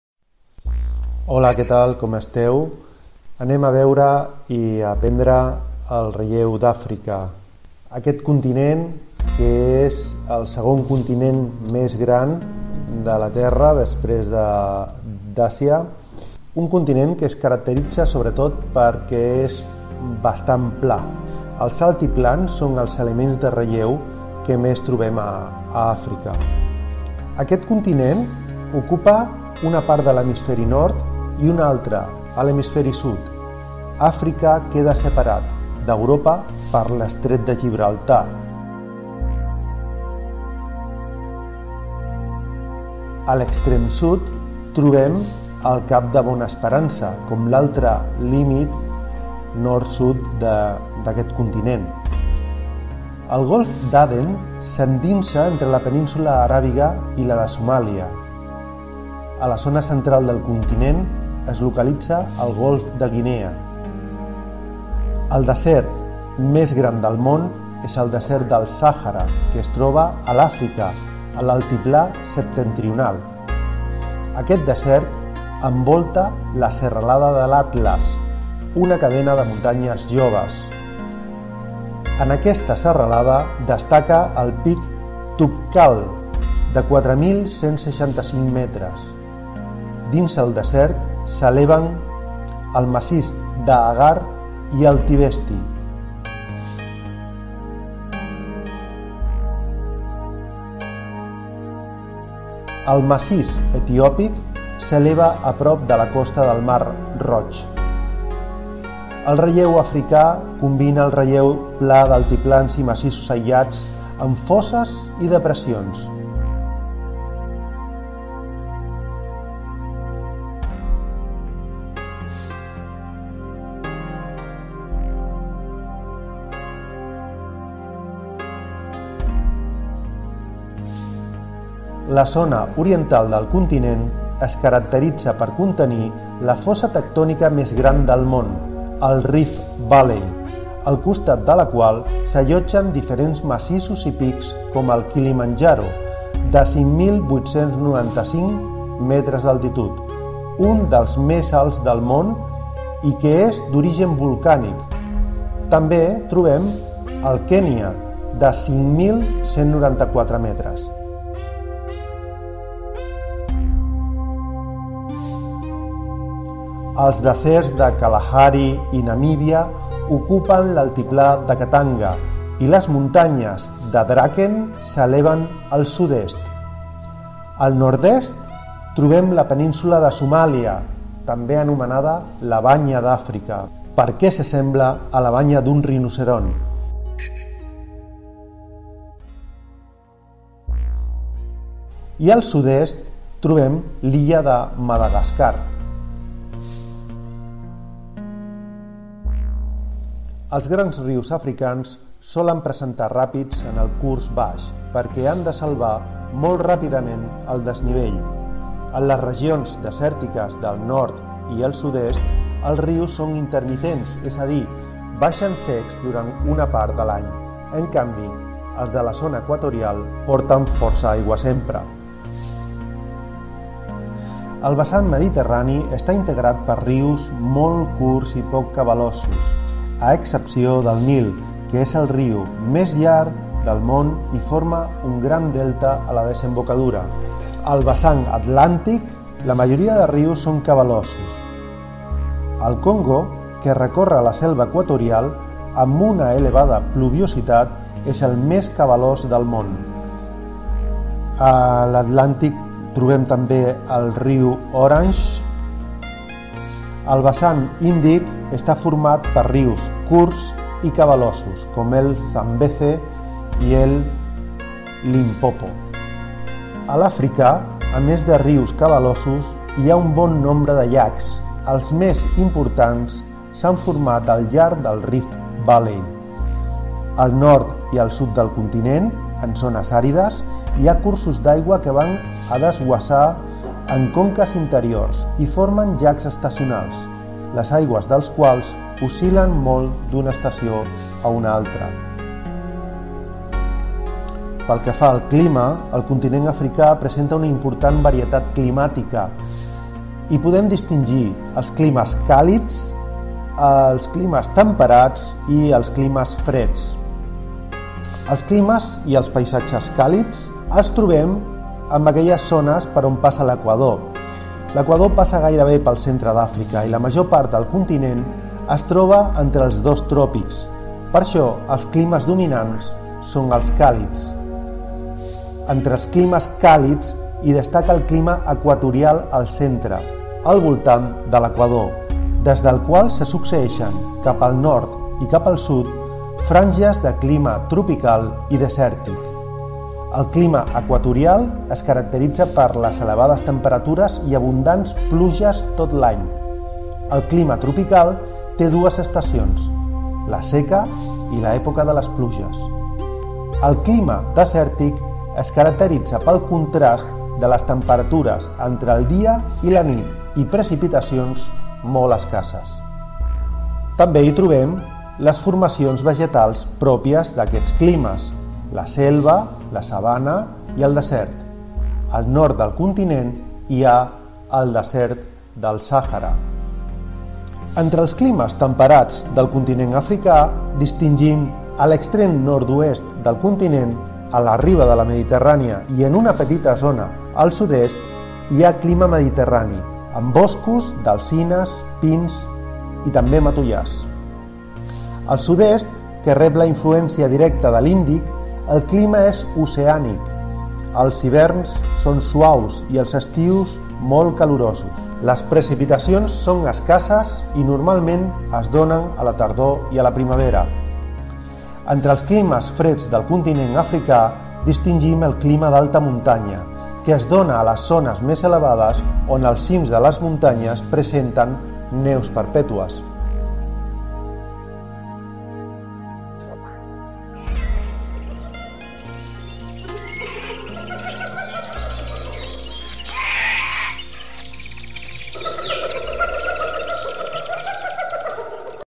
el relleu d'Àfrica
Video que explica de forma molt resumida el relleu, la hidrografia i el clima d'Àfrica